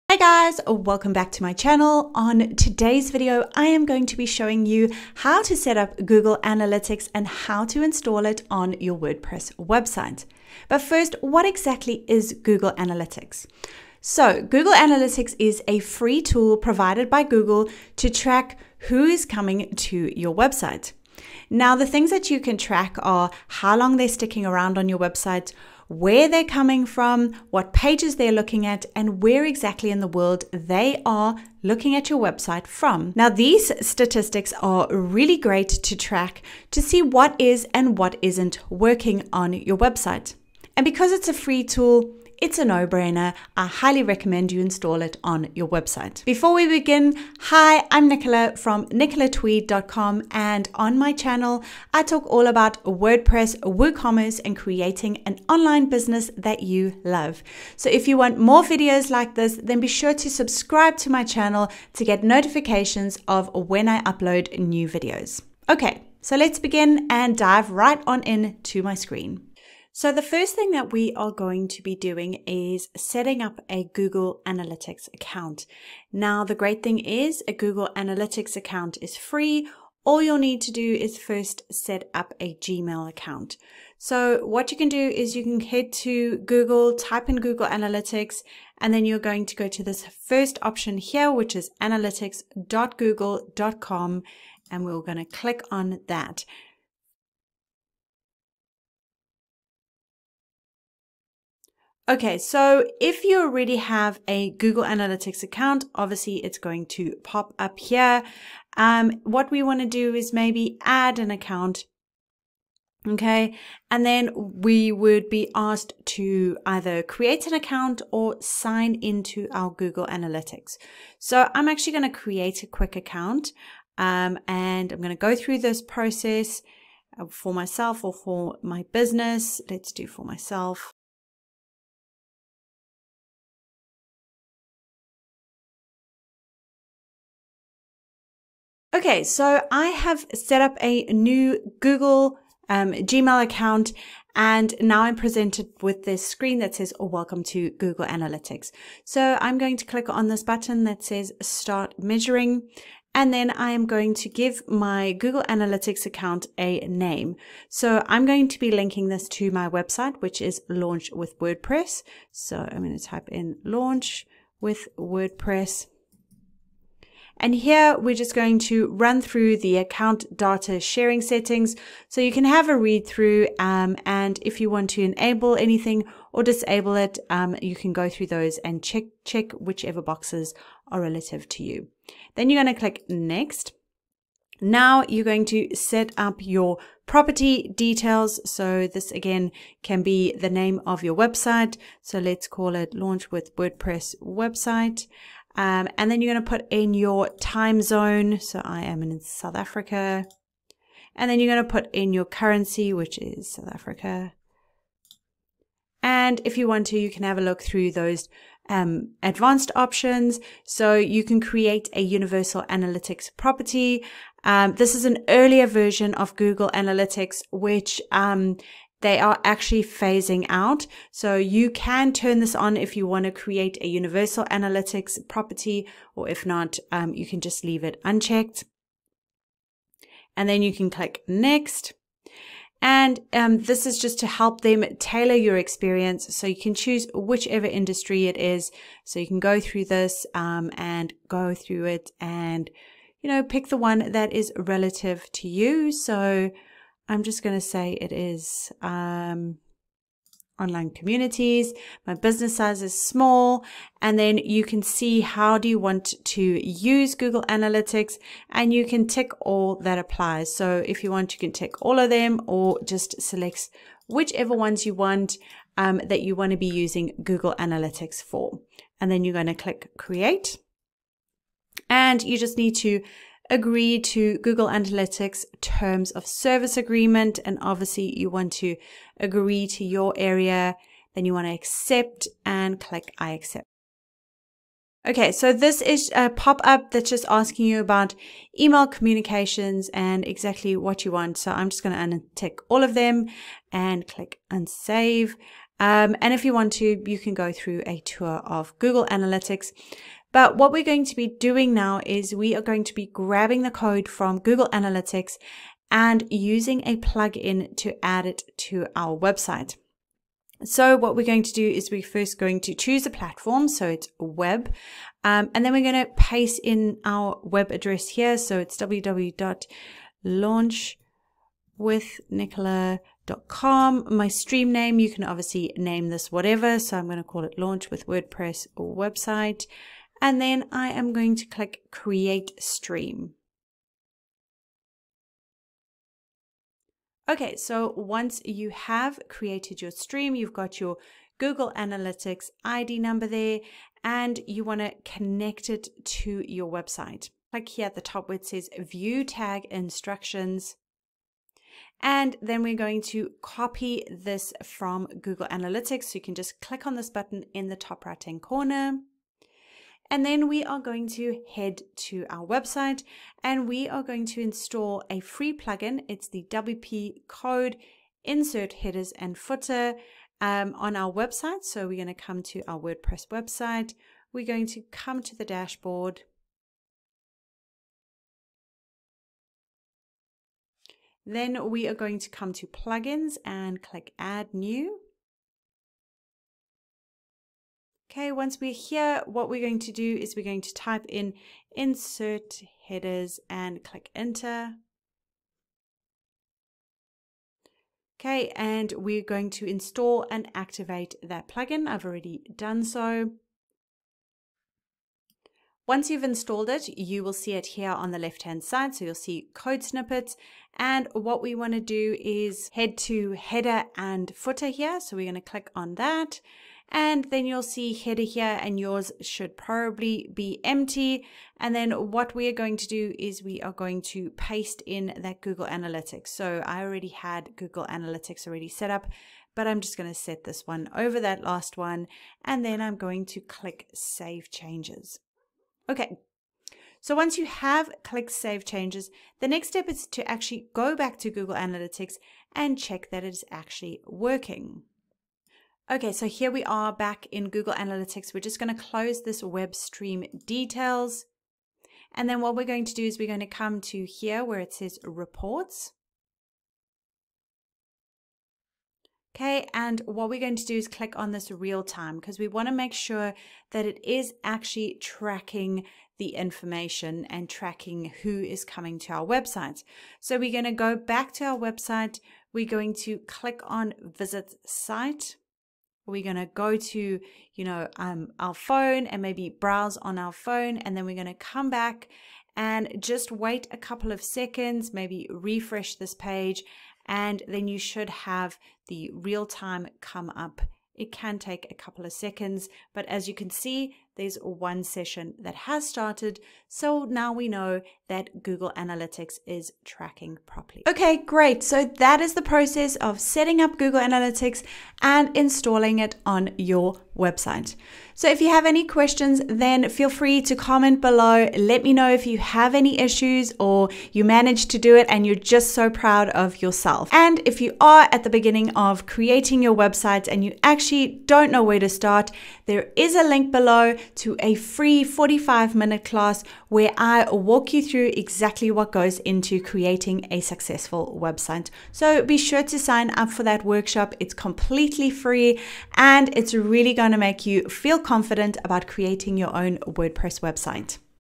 How to Install Google Analytics in WordPress in 10 mins
Google Analytics is a FREE tracking tool provided by Google to help you see who is coming to your website, what pages they are viewing, where they are coming from and how long they are sticking around for. It's a great tool to have on your website to see what is and isn't working on your site.

In this video, I show you how to create a Google Analytics account then grab the site tag to install on your WordPress website.

Learn how to crush your goals, attract your dream clients and build a stand out website with my FREE three step guide